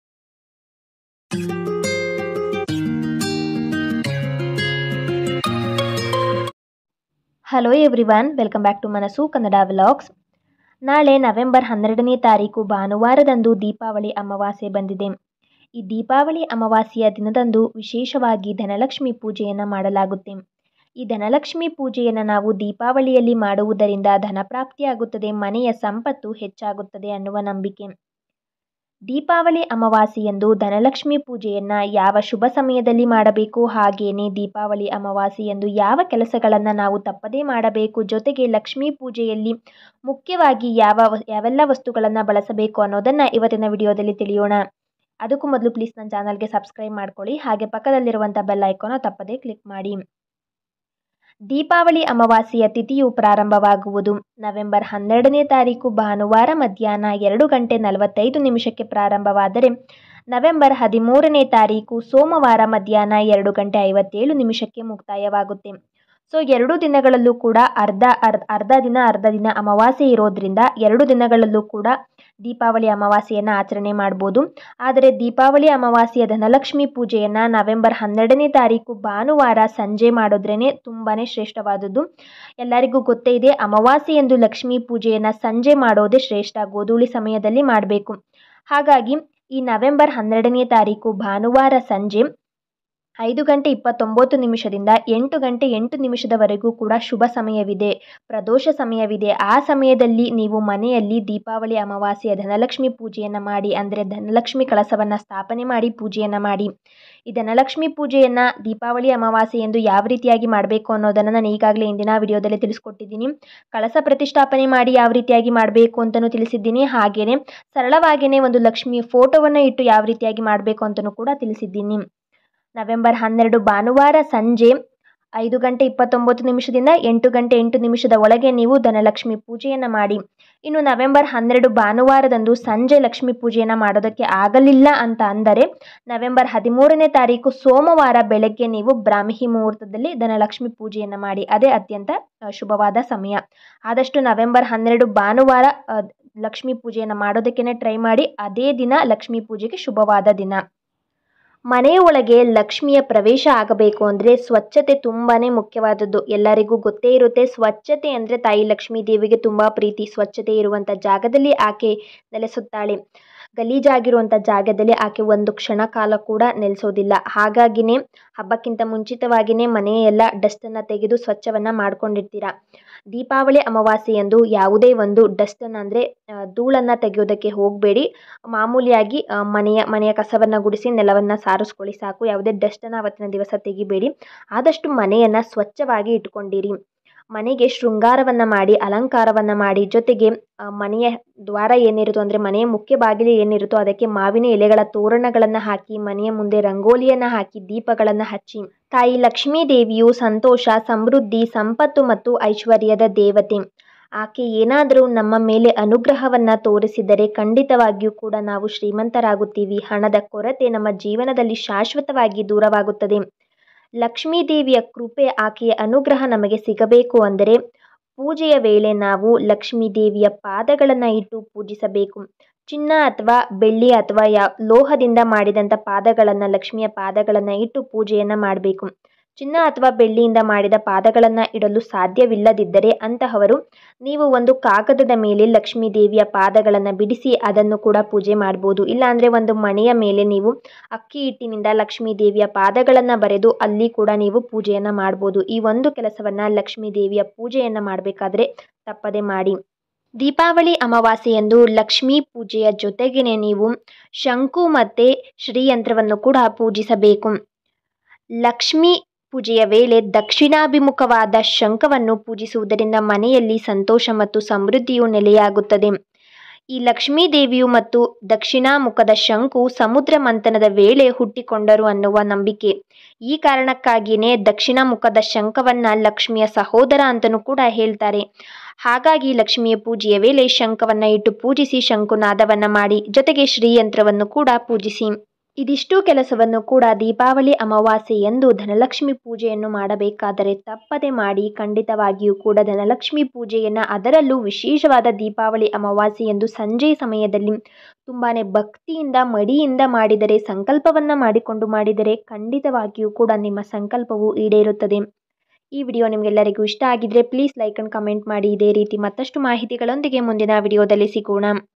ನಾಳೆ ದೀಪಾವಳಿ ಅಮಾವಾಸ್ಯೆ ಲಕ್ಷ್ಮಿ ಪೂಜೆ ಮಾಡಲು ಶುಭ ಸಮಯ ಯಾವುದು? ನಾಳೆ ಈ ಕೆಲಸ ಮಾಡಿ ಹಣದ ಕೊರತೆ ದೂರವಾಗುತ್ತದೆ
ನಾಳೆ ದೀಪಾವಳಿ ಅಮಾವಾಸ್ಯೆ ಲಕ್ಷ್ಮಿ ಪೂಜೆ ಮಾಡಲು ಶುಭ ಸಮಯ ಯಾವುದು? ನಾಳೆ ಈ ಕೆಲಸ ಮಾಡಿ ಹಣದ ಕೊರತೆ ದೂರವಾಗುತ್ತದೆ. @manassukannadavlogs9694